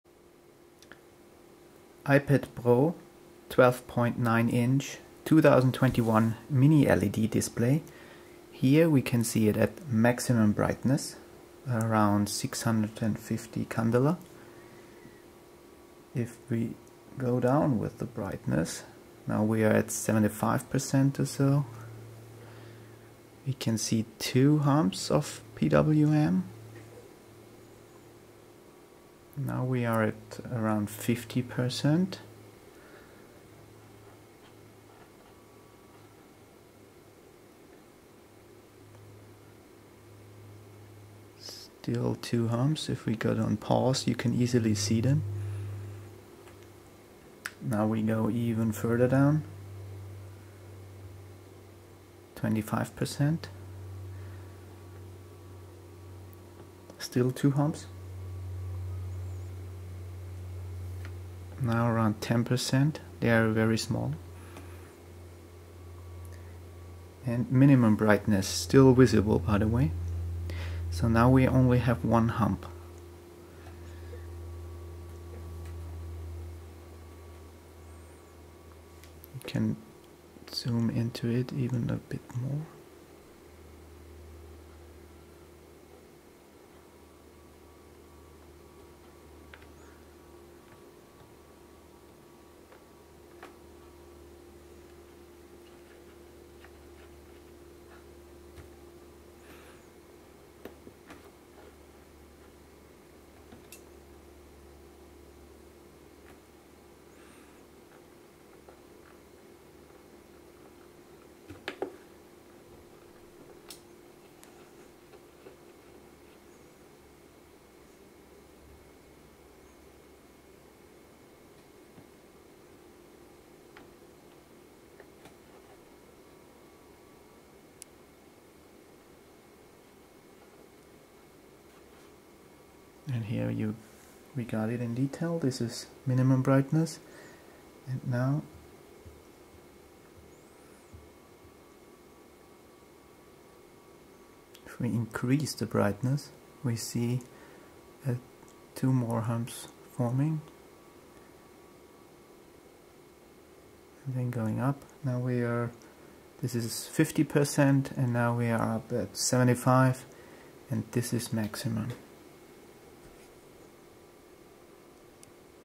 Apple iPad Pro 12.9-inch 2021 M1 PWM of the Mini LED Display
The new mini LED display uses PWM with a high frequency of 19 kHz to regulate the brightness. Even at 100% the screen uses PWM (at measured 620 nits). This means, very sensitive persons should not use the iPad for long periods. This measurement is part of our extensive review of the 2021 Apple iPad.